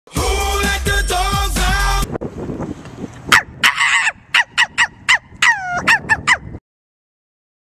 Girls Barking 2
We were up in Vermont and found a cool barker girl.  Girls barking at UVM in the Green Mountain State.